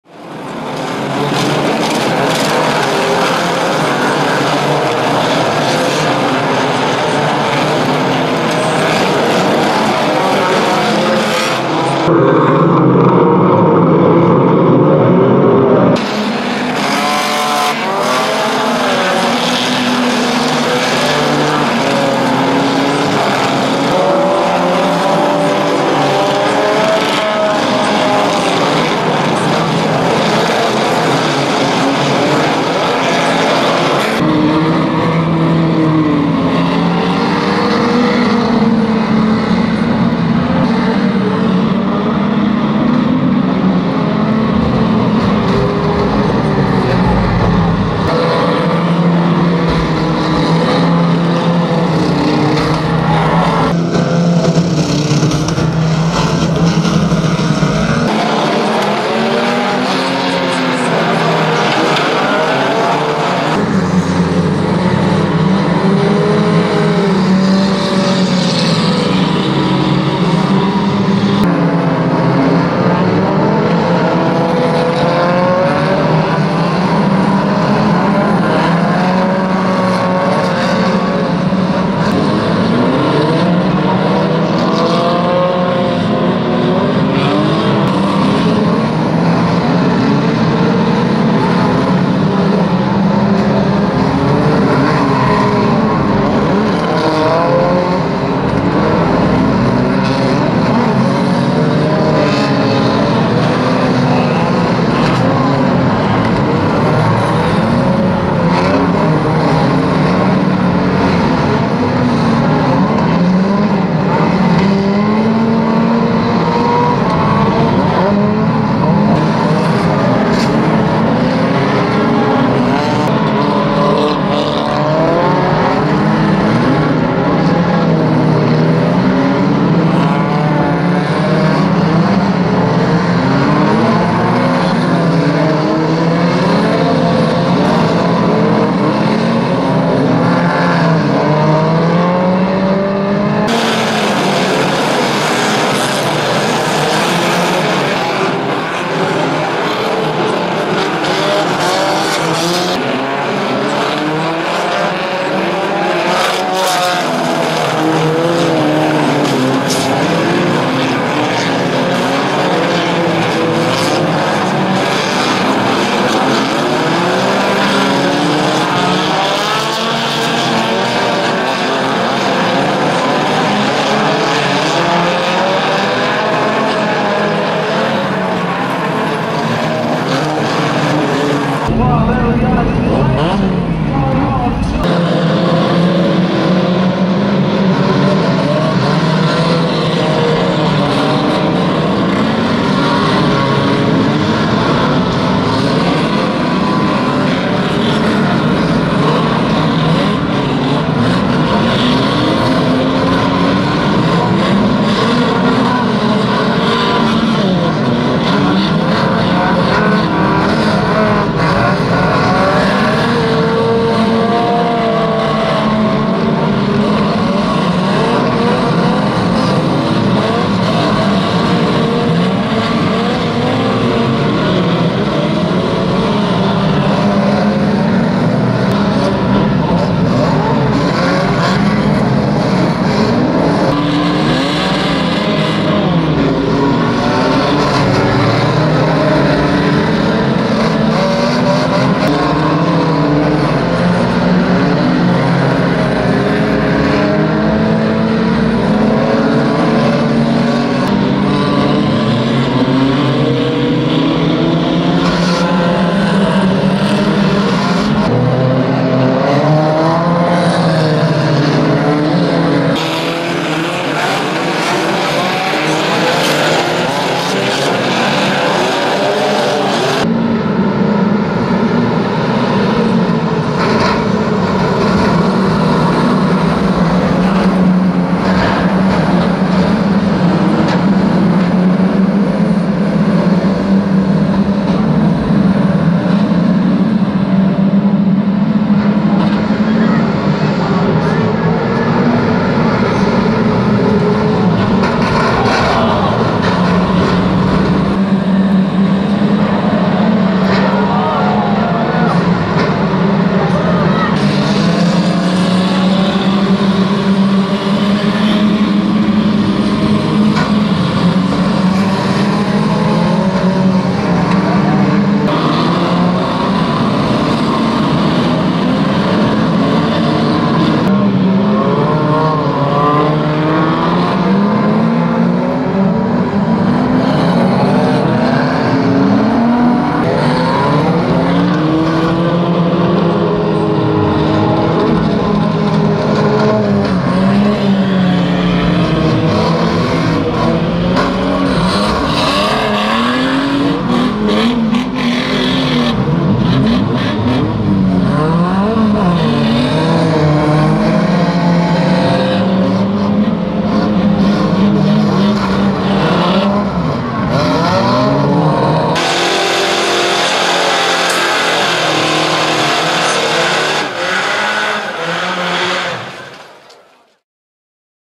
Kings Lynn 2023 Unlimited Bangers East Anglian Impact Videos Preview
A selection of crash action from our 2 camera mix of the 2023 East Anglian Championship for Unlimited Bangers. Some hard hits can be seen and also features some brutal Saloon Stock Car action and Junior Bangers. The full DVD production features every race in full with live race commentary, pit and wreck shots and action replays.